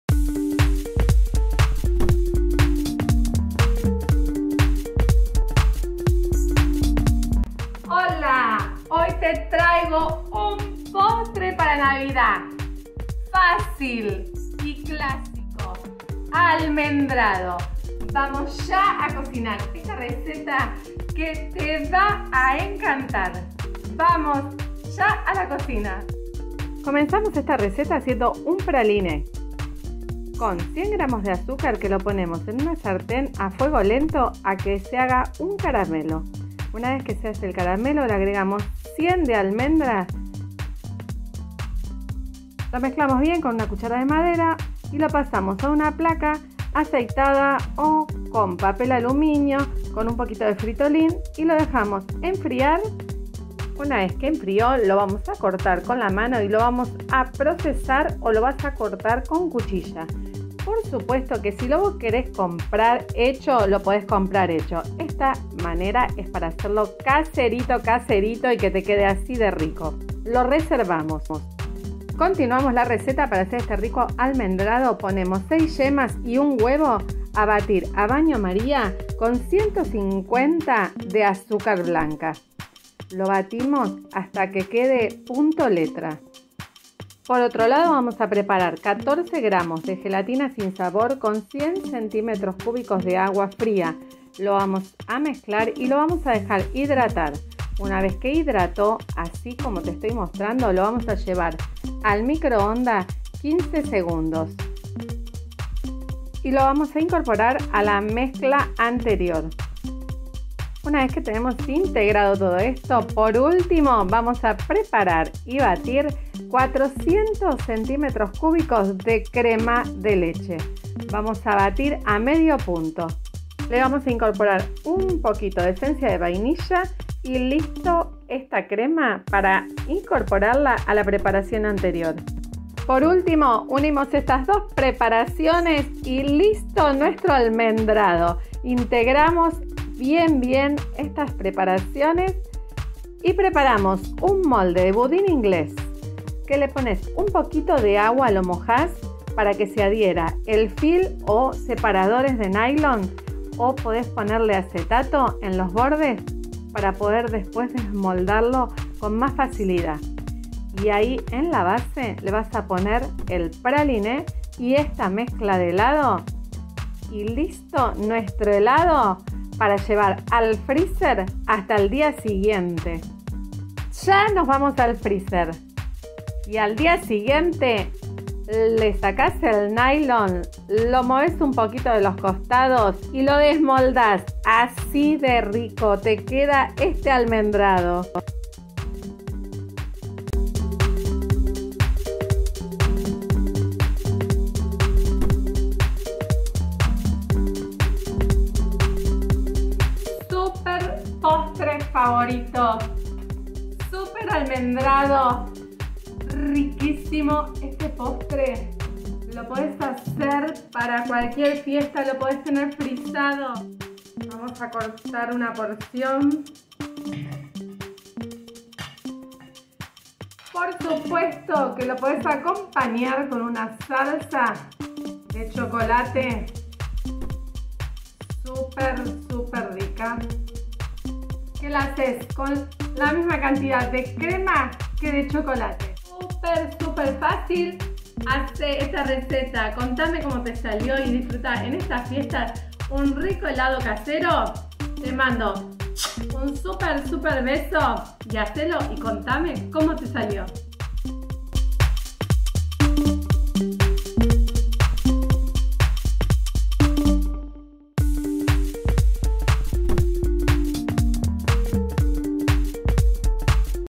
HELADO ALMENDRADO CASERO!!! IMPERDIBLE!
PRALINE
100 AZUCAR
100 ALMENDRA
HELADO
6 YEMAS
1 HUEVO
150 AZUCAR
14 GELATINA SIN SABOR 100 AGUA
ESENCIA DE VAINILLA
400 CREMA LECHE